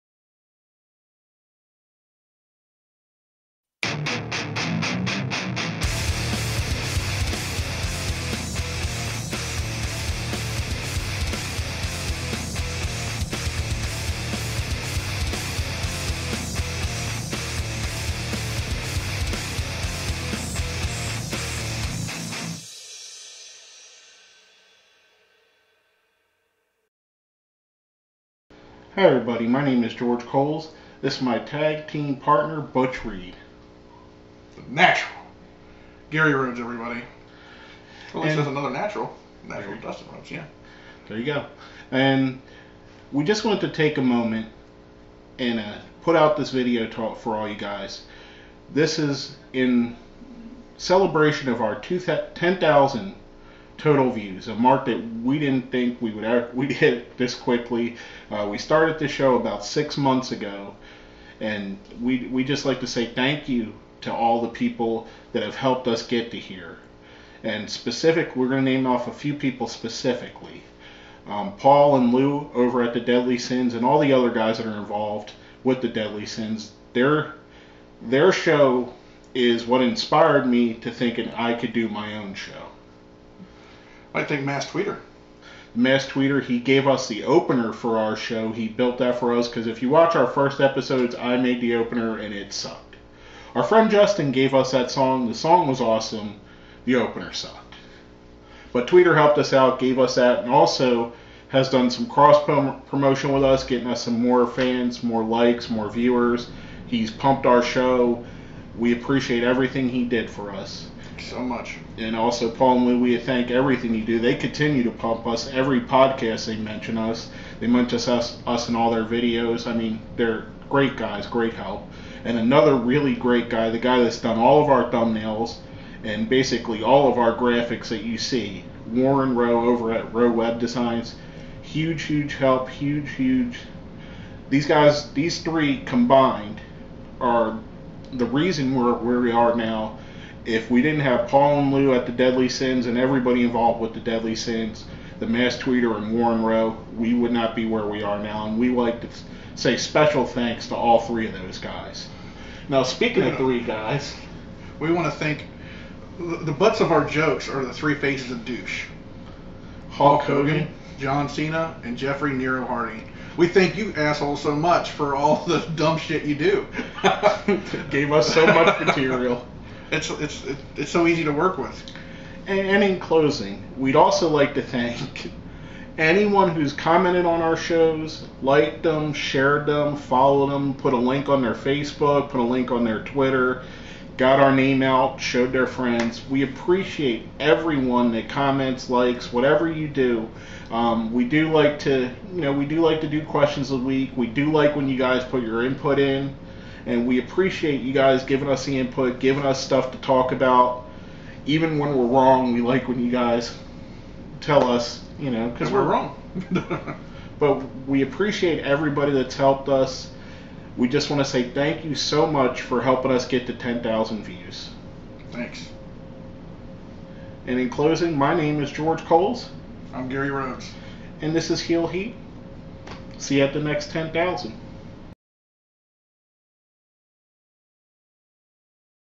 10,000 Views!!!!! Thank you for your support!!!!!
A quick video talking about reaching the 10,000 view milestone